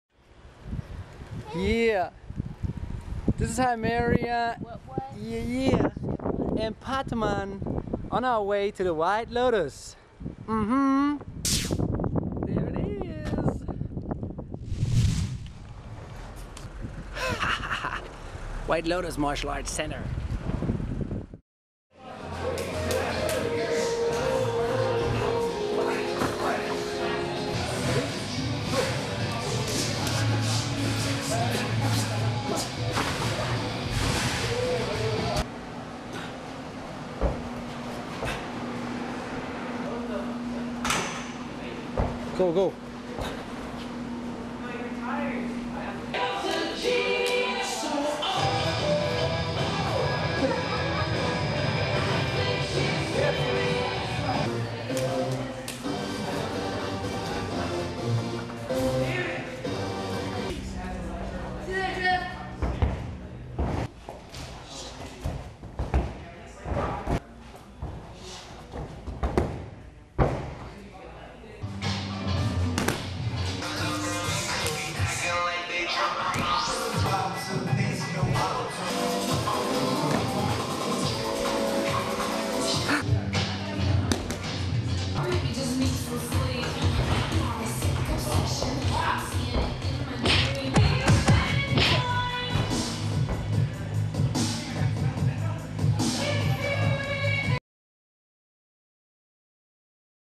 Meme & Pata
Short trainings video from Meme & Pata in the white lotus martial arst center. enjoy !!